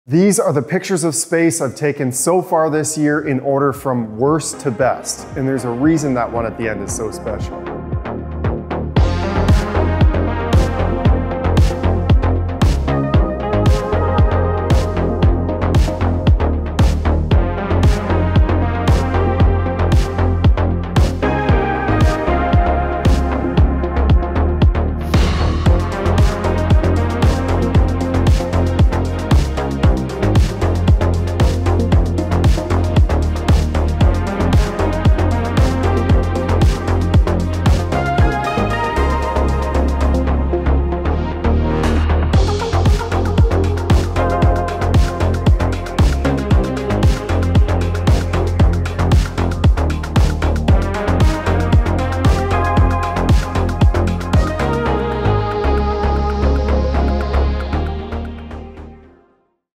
Pictures of Space I Took
These are the pictures of space I've taken this year so far!